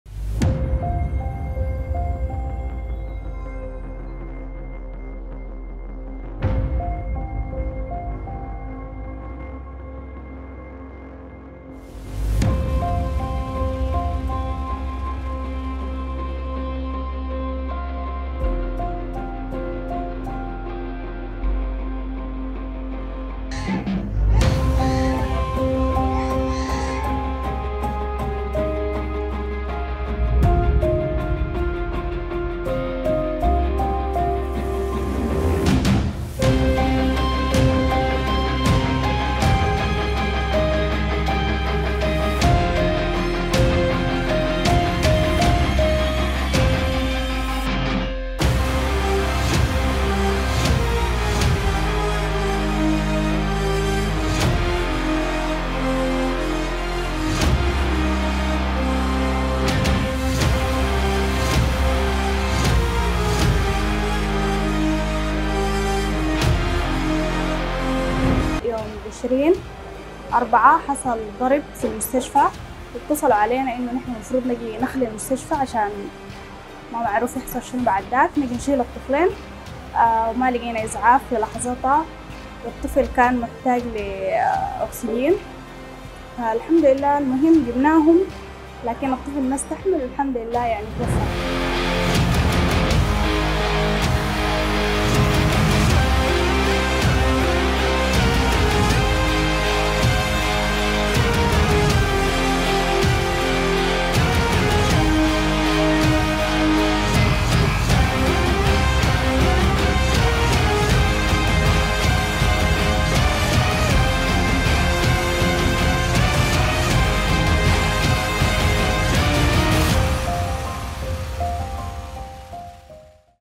أم سودانية تفقد أحد رضيعيها التوأم.. قُصف المستشفى ففرت بهما للشارع
فرت بهما في الشارع فتوفي بسبب نقص الأكسجين.. أم سودانية تفقد أحد رضيعيها التوأم بعد قصف استهدف مستشفى في